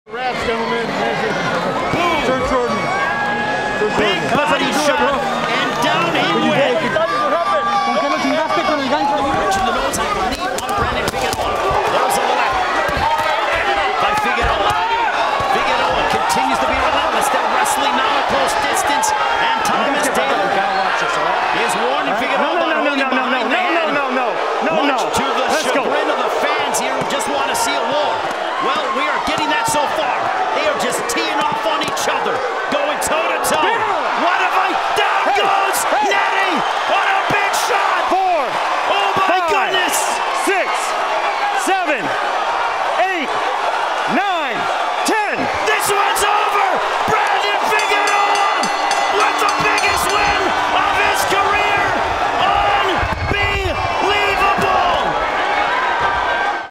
Brandon Figueroa Brings RELENTLESS Pressure
When you're up against Brandon Figueroa's RELENTLESS pressure, it's only a matter of time before he lands the lethal blow.

Figueroa returns THIS SATURDAY, March 4, 2023, against Mark Magsayo, live on Showtime Boxing